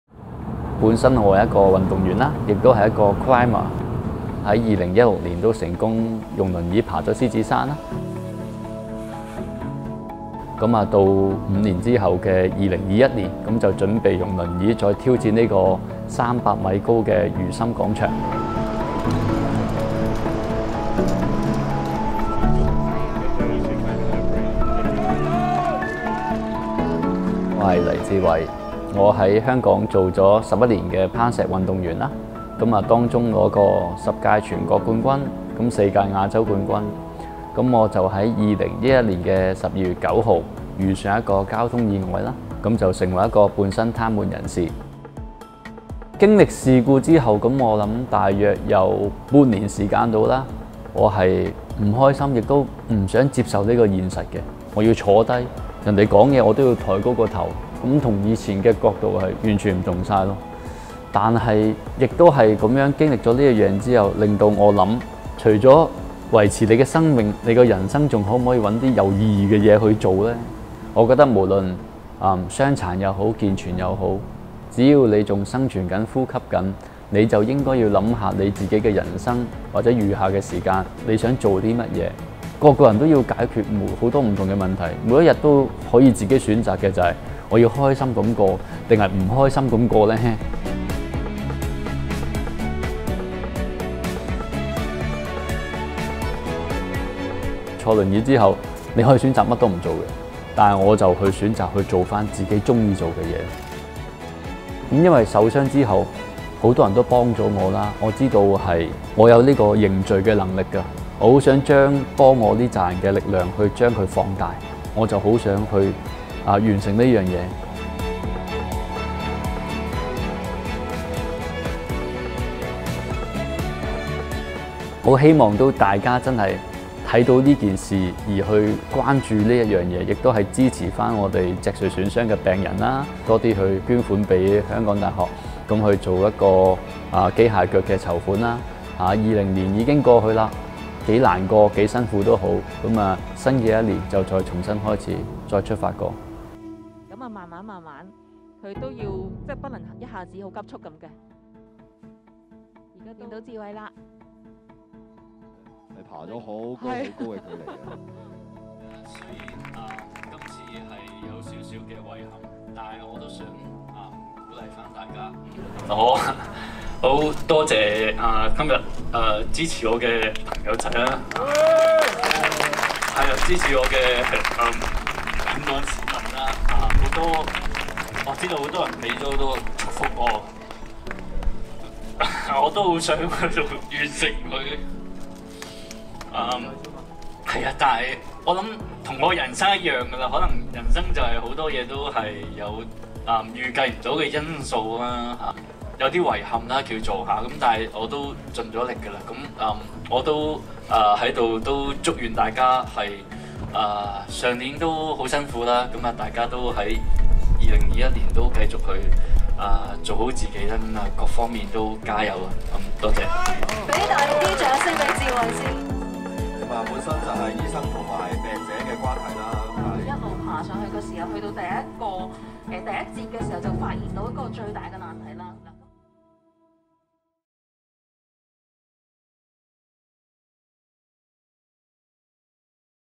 Hong Kong wheelchair climber Lai Chi-wai attempts to scale 320-meter skyscraper
Lai Chi-wai was once one of the world’s best climbers, until a traffic accident left him a paraplegic. Although Lai lost the use of both legs, he did not give up his passion. In 2016, Lai climbed up Lion Rock in a wheelchair using only his upper body strength. Five years later, he attempted to climb the 320-metre Nina Tower in Tsuen Wan, hoping to help lift people’s spirits during the Covid-19 pandemic.

(Photo: SCMP / Dickson Lee)